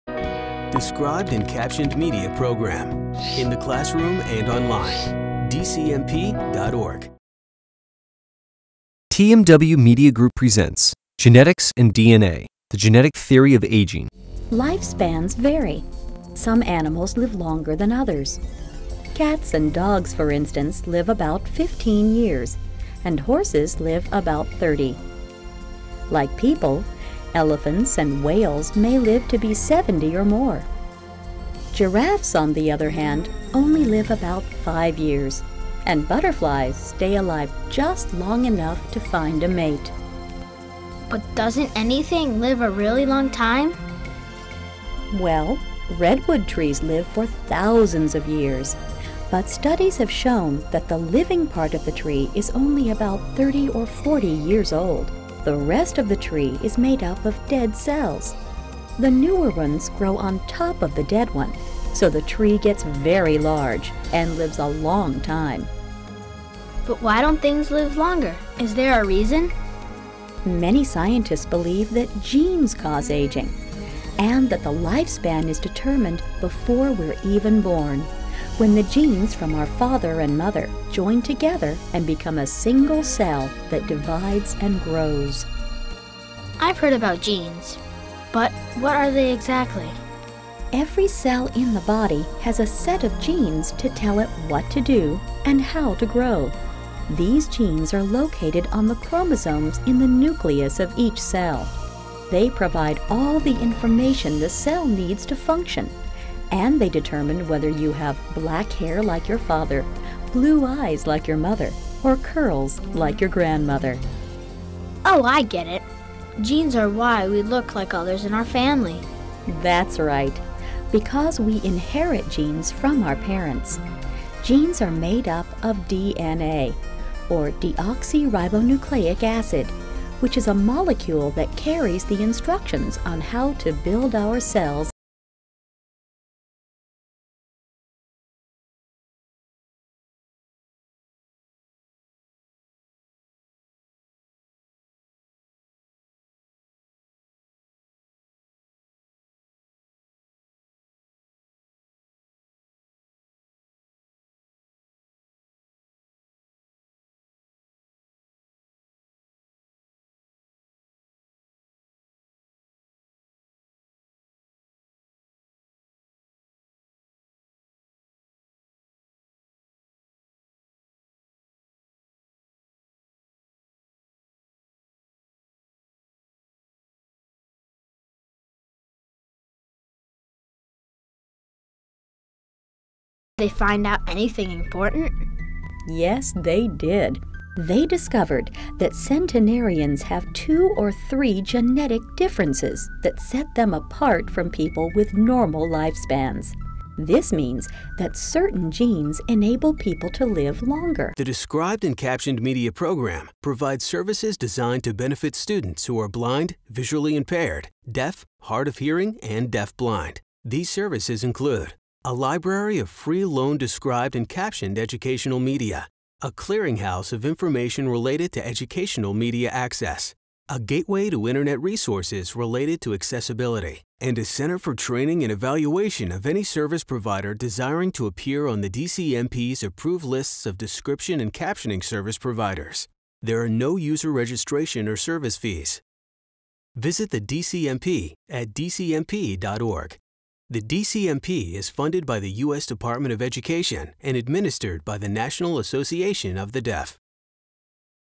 Genetics & DNA: The Genetic Theory Of Aging (Accessible Preview)
No organism lives forever. The length of time animals and humans live is influenced by their genes. Scientists have made astonishing discoveries concerning the role of genetics in determining life span, and this holds promise of extending the lives of animals and humans. Explains genetics, DNA, and genetic theories of aging. Illustrates the genetic processes behind cellular aging, and shows how genes affect life span. Discover the reasons why cells age and why a certain enzyme can effectively turn back the hands of our "biological clock."

Producer/Distributor: TMW MEDIA GROUP
Production Year: 2012
Grade Level: 7-12